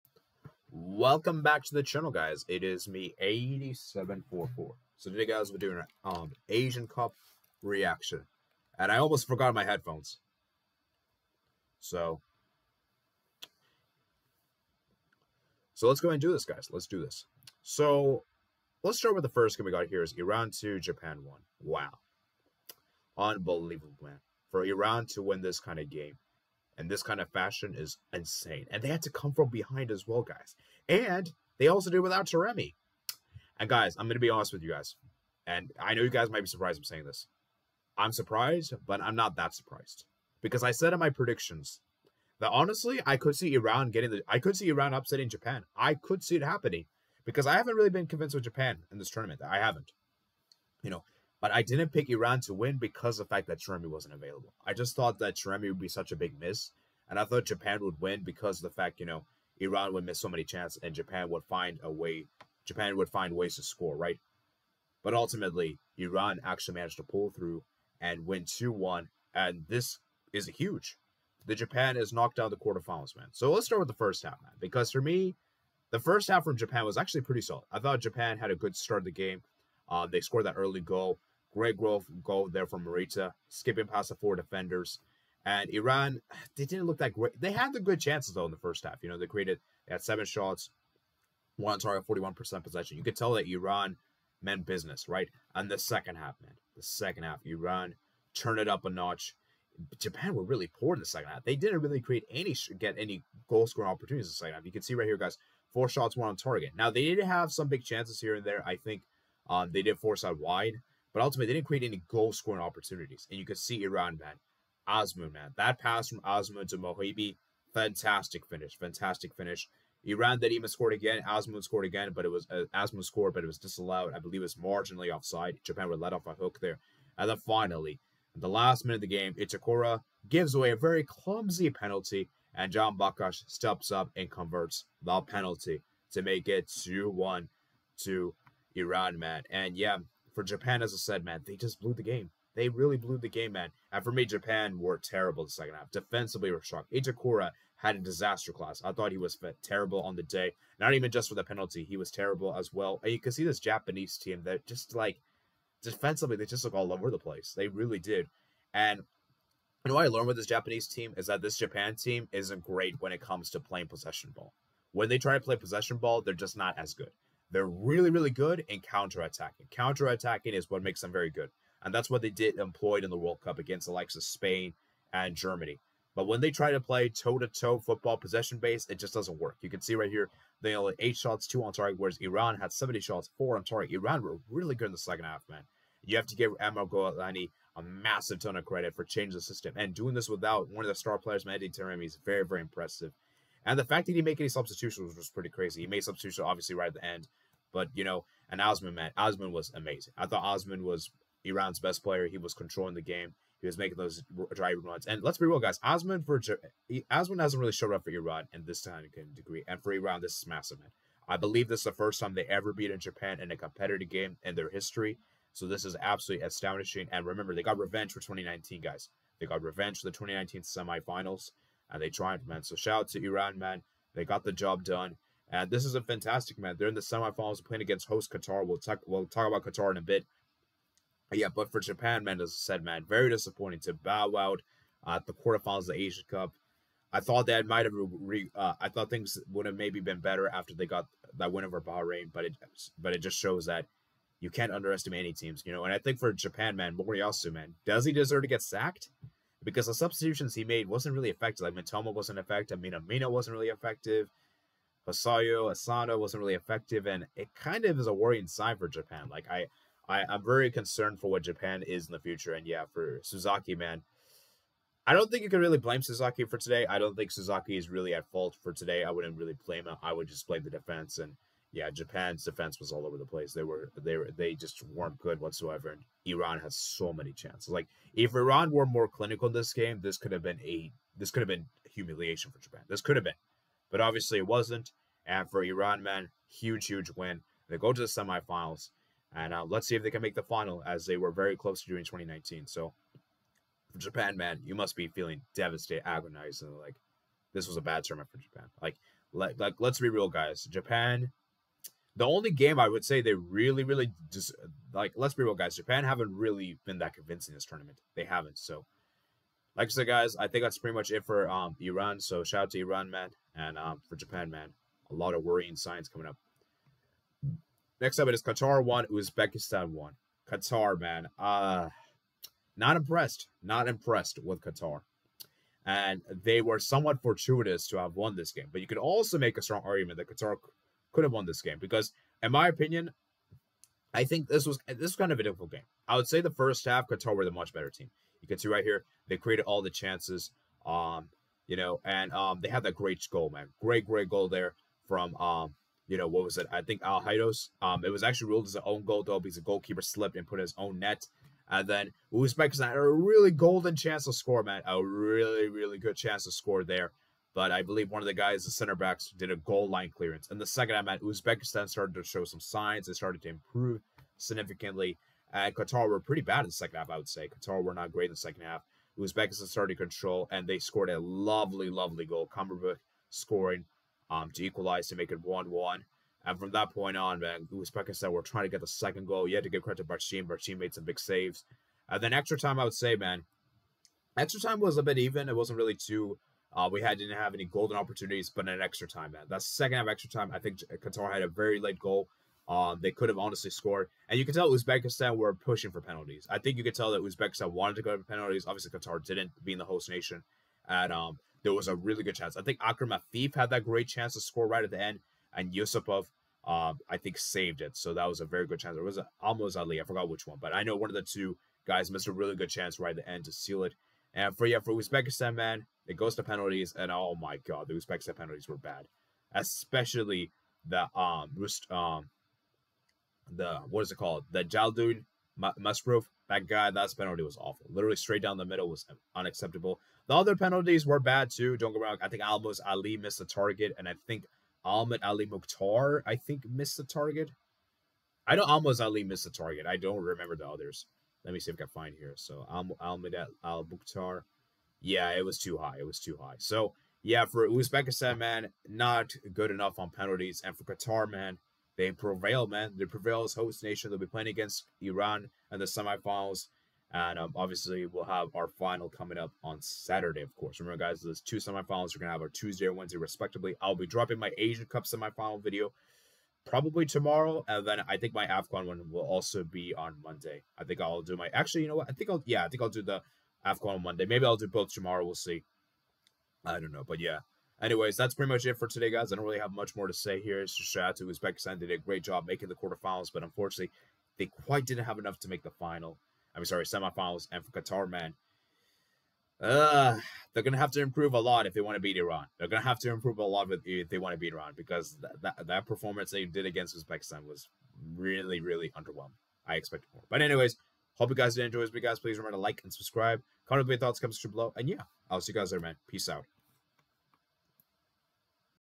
IRAN BEATS JAPAN AND QATAR HOST NATION ADVANCE TO THE SEMI-FINALS | ASIAN CUP REACTION
Timestamps:
0:00 - Intro
0:20 - Iran vs Japan
5:00 - Qatar vs Uzbekistan